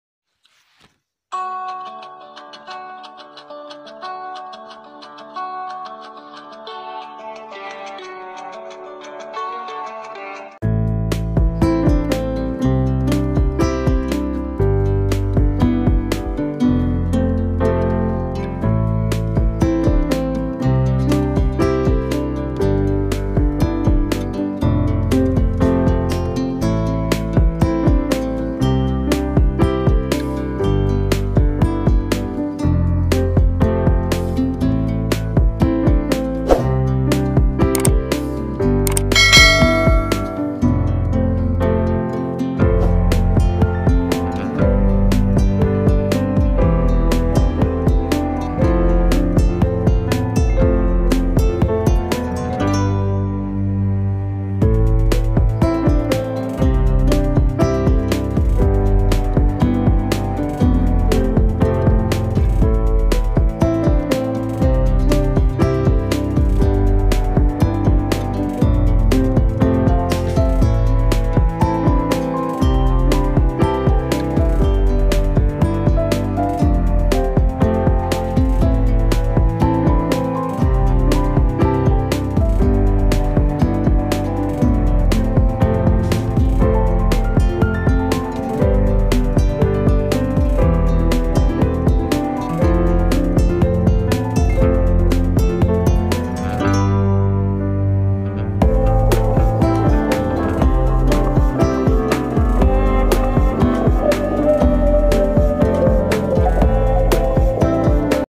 4th Unit Test 2023 Class 6 Maths Question Paper চতুৰ্থ গোট মূল্যায়ন ২০২৩ প্ৰশ্নকাকত
4thUnitTestQuestionPaperClass6 Class6UnitTest2023 BiswajitBorahEducation Class 6 4th Unit Evaluation Question Paper Subject Maths